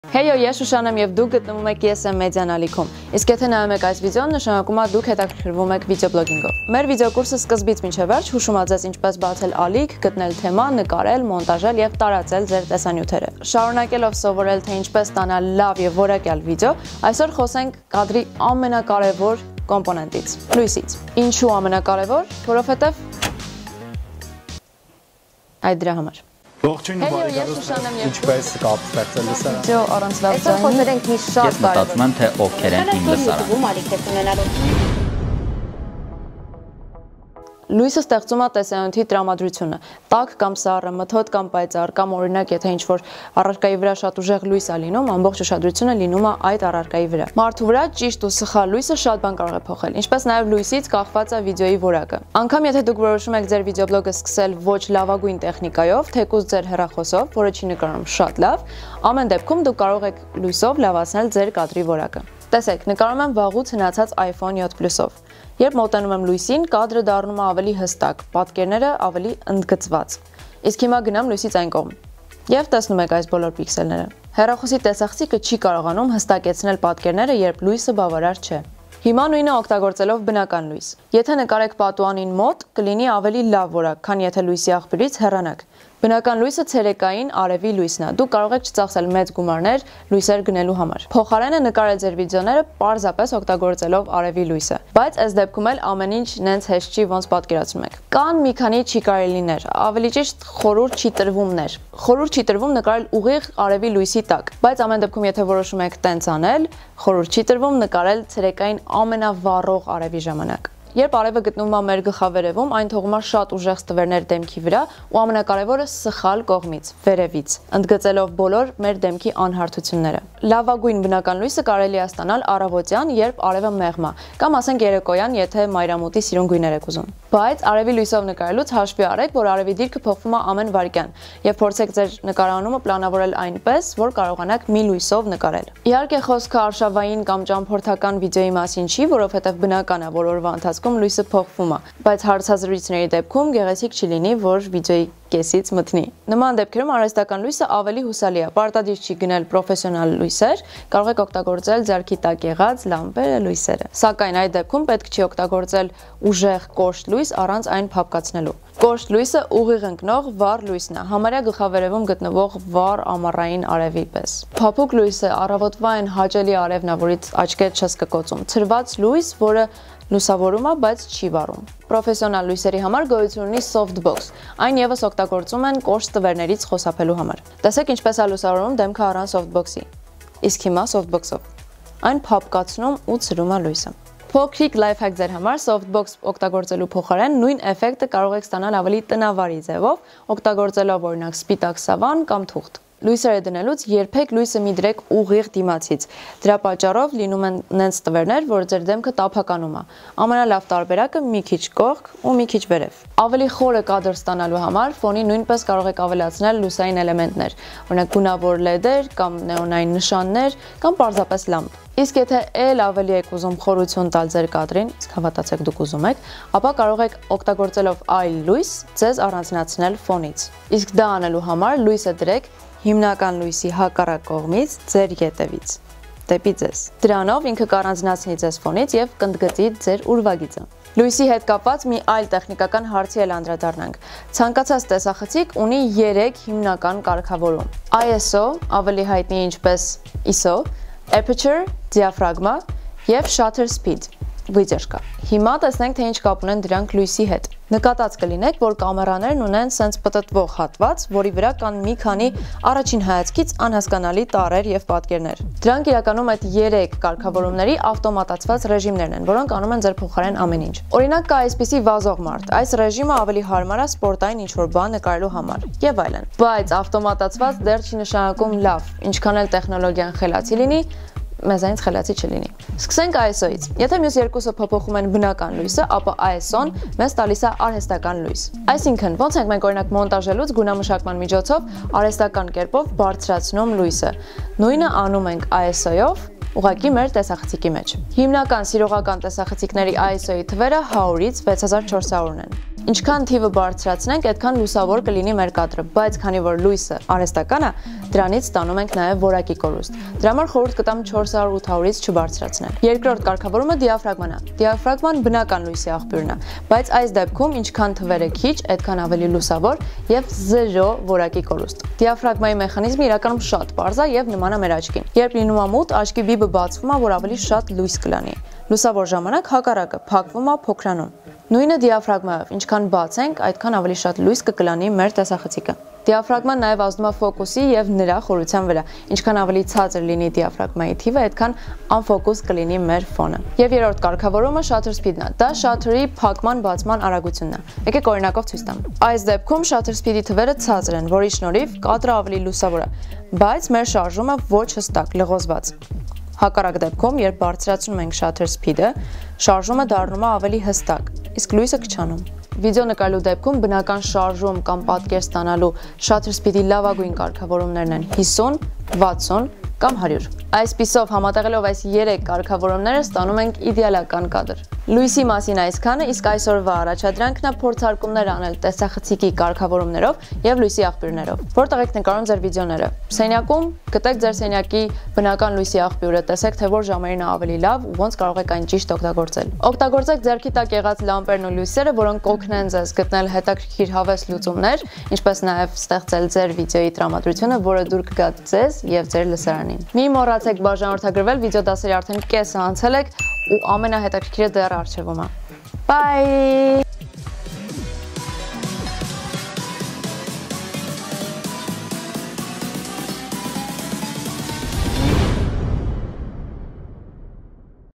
Տեսանյութի լուսավորությունը
Լույսը տեսանյութի տրամադրությունն է ստեղծում՝ տաք կամ սառը, մթոտ կամ պայծառ, ուրախ կամ խորհրդավոր․․․  Լուսավորությունից կախված է նաև վիդեոյի որակը։ Անգամ եթե դուք որոշում եք սկսել ձեր վիդեոբլոգը ոչ լավագույն տեխնիկայով, թեկուզ ձեր հեռախոսով, որը չի նկարում շատ լավ, ԱՄԵՆ ԴԵՊՔՈՒՄ, ճիշտ լույսի շնորհիվ դուք կարող եք լավացնել որակը։ Շուշանը կպատմի գաղտնիքների մասին։

Սա վլոգինգի օնլայն կուրսի 7-րդ վիդեոն է։ Չմոռանաս դիտել նաև մյուսները։